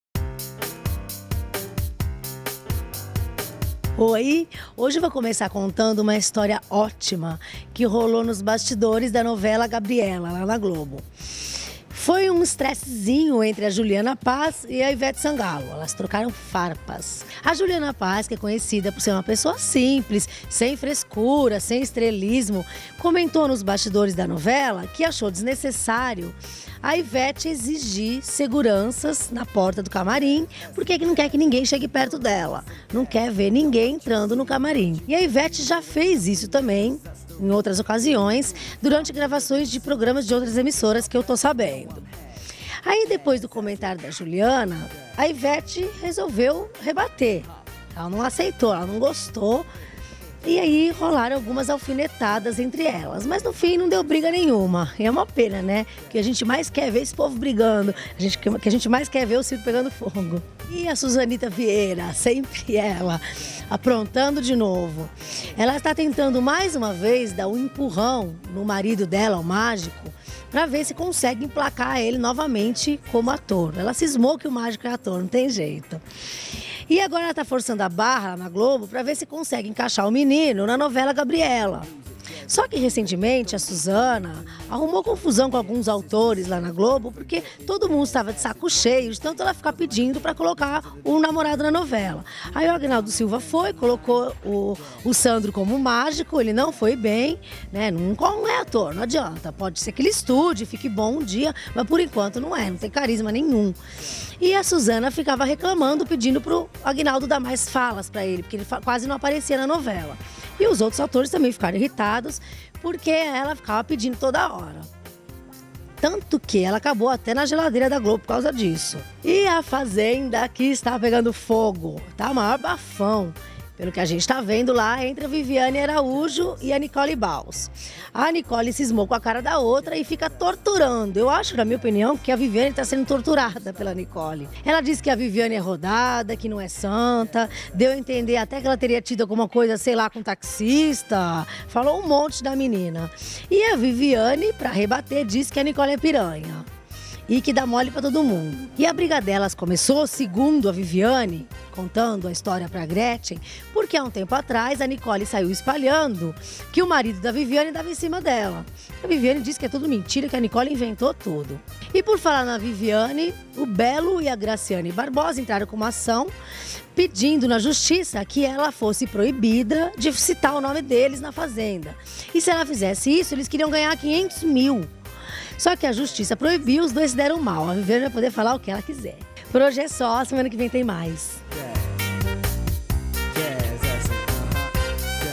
Juliana Paes e Ivete Sangalo se estranham nos bastidores de Gabriela
Juliana Paes comentou nos bastidores da novela Gabriela que acha desnecessário a musa do axé ter seguranças na porta do camarim durante as gravações. Ivete não gostou do comentário e as duas musas andaram se alfinetando. Veja também Suzana Vieira volta a forçar a barra para arrumar trabalho de ator para o marido e na Fazenda o clima esquenta entre Nicole Bahls e Viviane Araújo.Veja também: Ivete Sangalo e Juliana Paes roubam a cena na Bahia